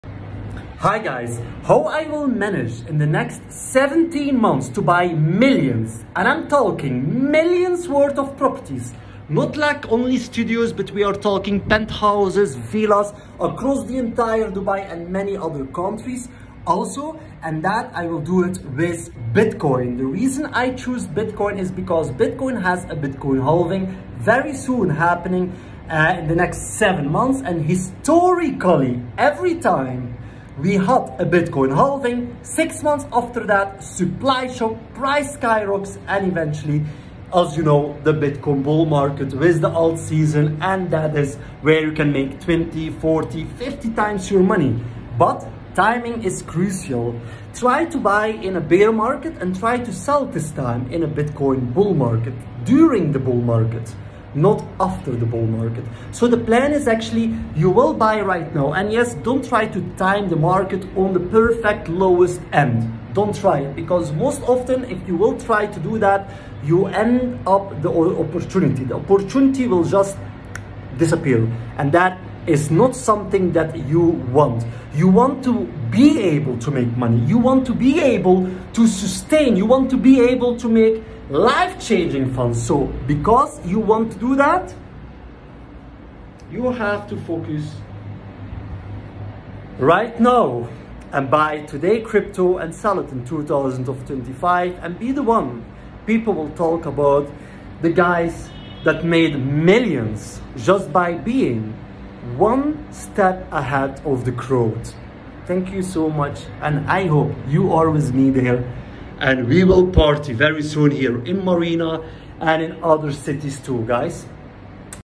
How to Buy Millions in Property with Bitcoin in 17 Months
In this video i will share with you my secret how i’m making money and how you can learn from my experience.